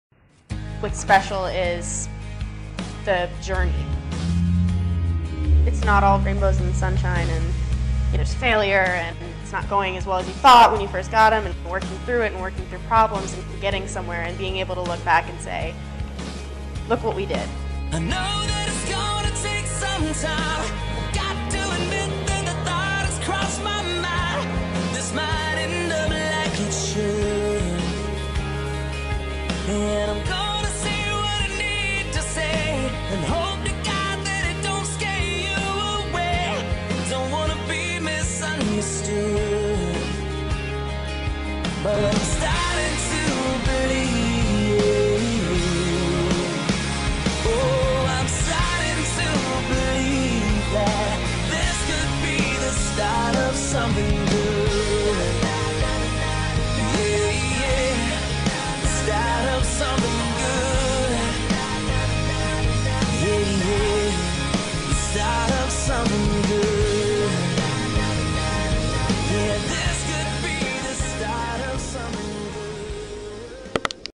This is Gunna Take Some Time..♥
Awh Just love this quote! Had some new clips of ponies, and teached Poppy so Smile!
Fun time making this video!! :D

thank you equially for putting the tutorial on youtube it worked!!

Also a  big thank you to xshowjumperx for making the quote!!